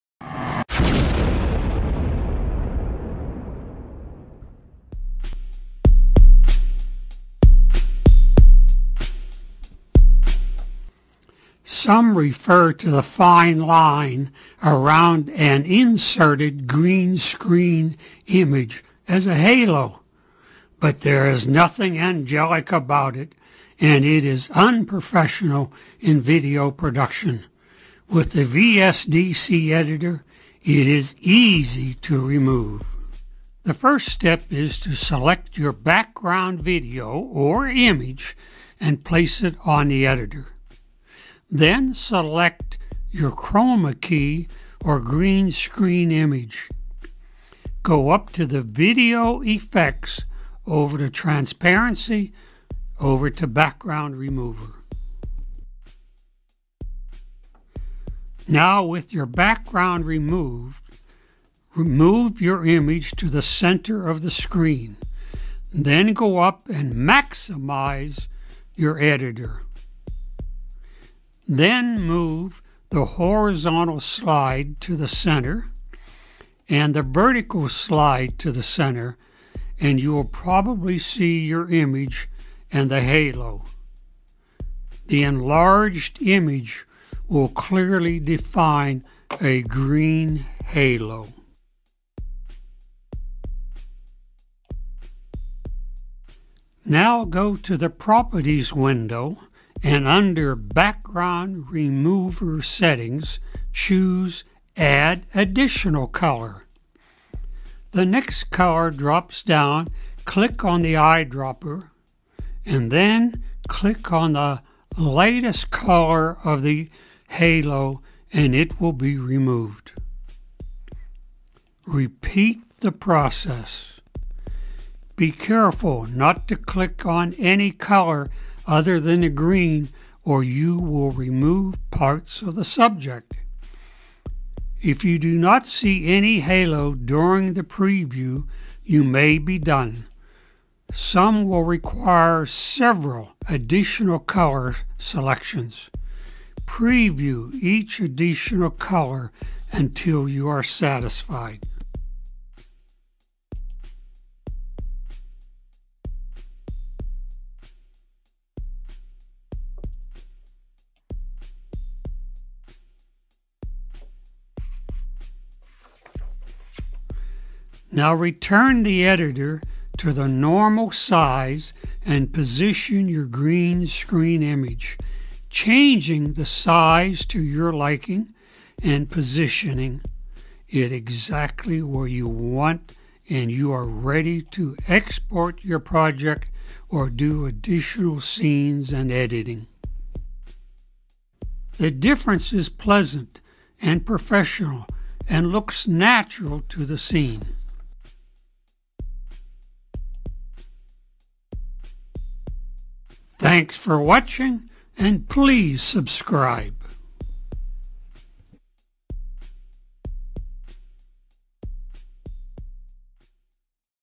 REMOVING HALO AROUND GREEN SCREEN OBJECTS
How to remove the Halo around Green Screen or Chroma-Key objects in easy steps.  The editing process is simple with the VSDC Editor and the results are very professional. When you are finished your green screen image will look natural and blend with the background scene.